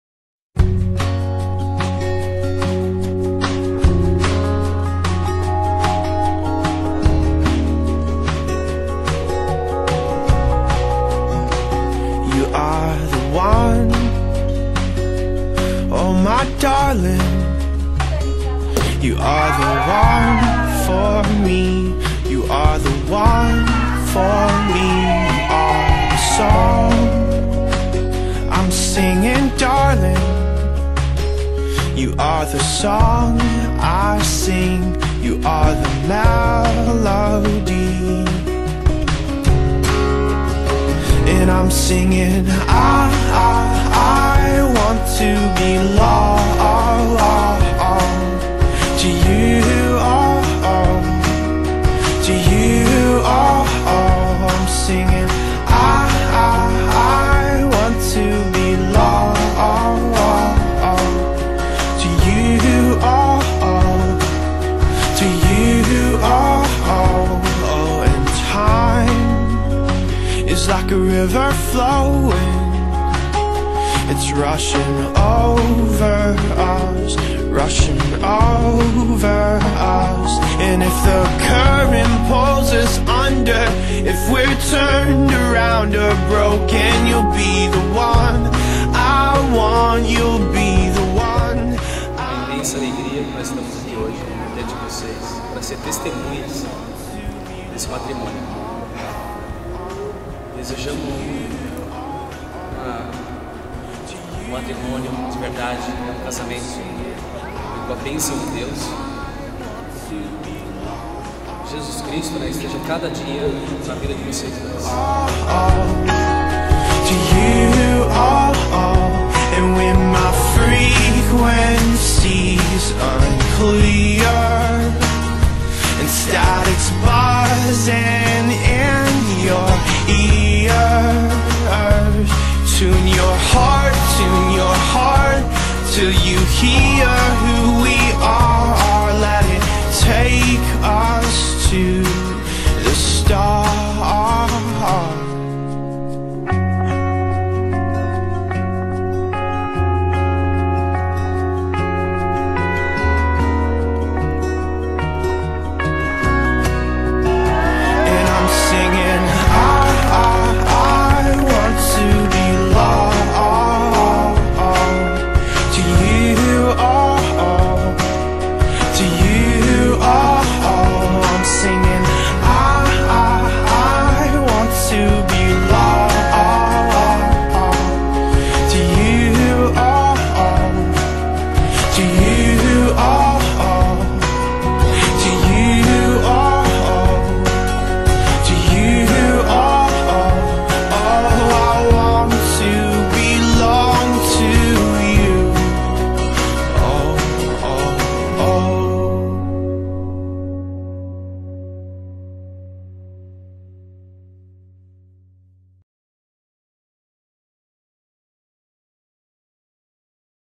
Lais e Eduardo - Clipe Final
Um resumo do lindo casamento da Lais e do Eduardo, espero que gostem!!!